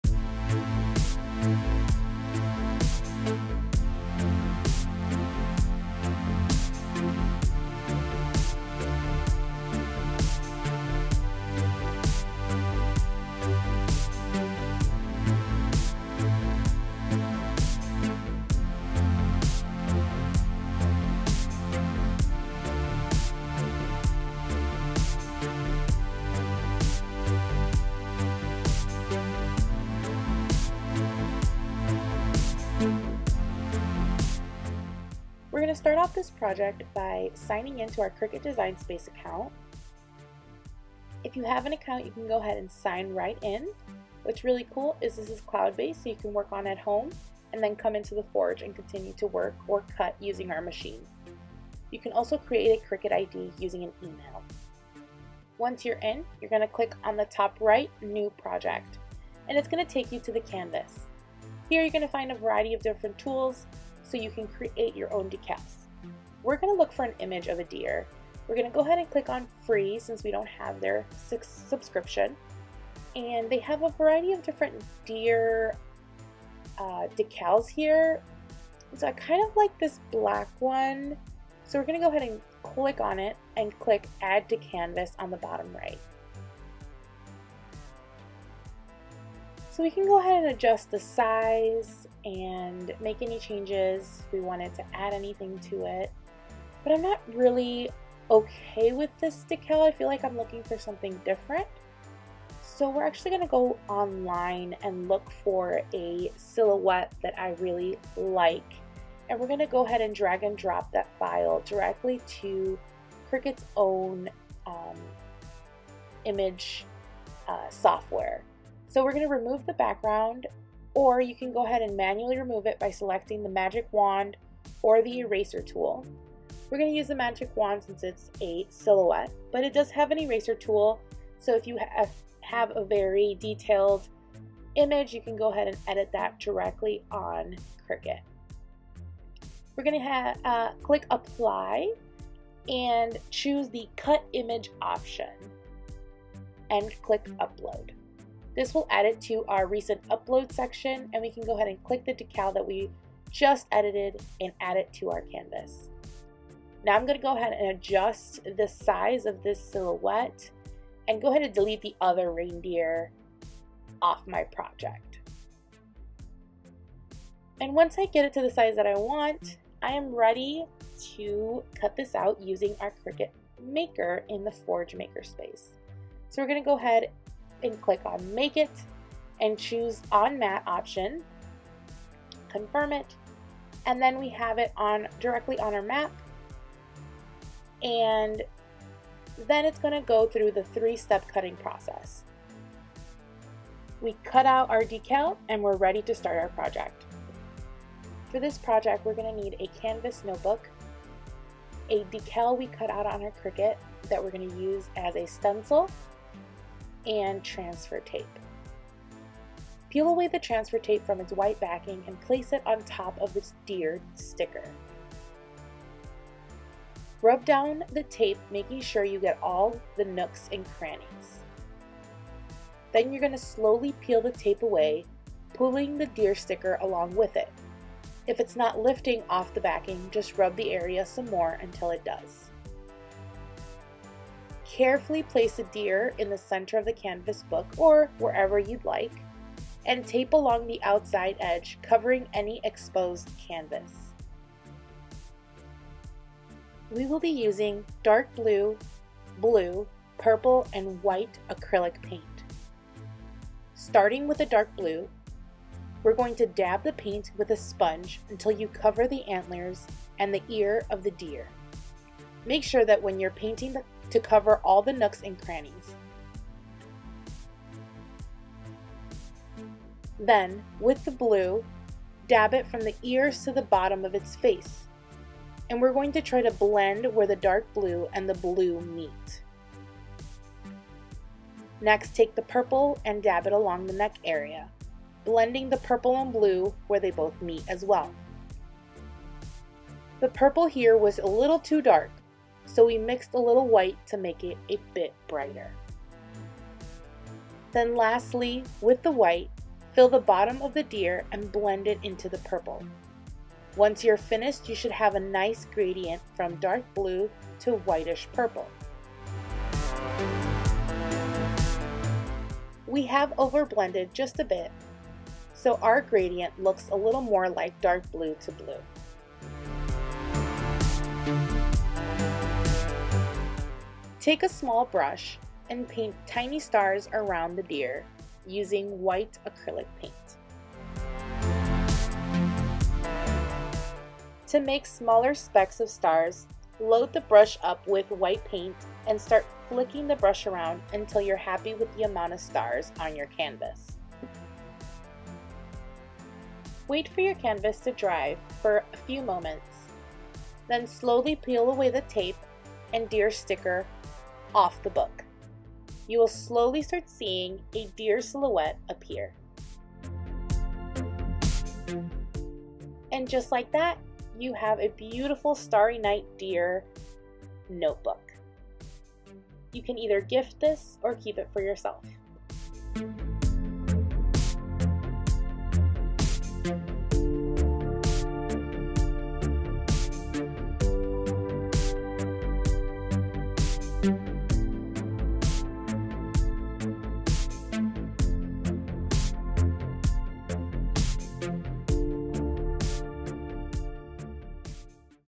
12 Days of Making : Starry Night Deer Canvas Notebook
Day 4 of our 12 Days of Making Series!

What Forge provides:
- Cricut Maker
- Cricut Design Space on our Forge computers.

What is needed:
- Canvas Notebook
- Vinyl
- Transfer Paper
- Acrylic Paint
- Paint sponge
- Paint brushes
- Tape

Drop by Ela Public Library's Forge makerspace and check out other amazing things you can make using our equipment, kits, and materials.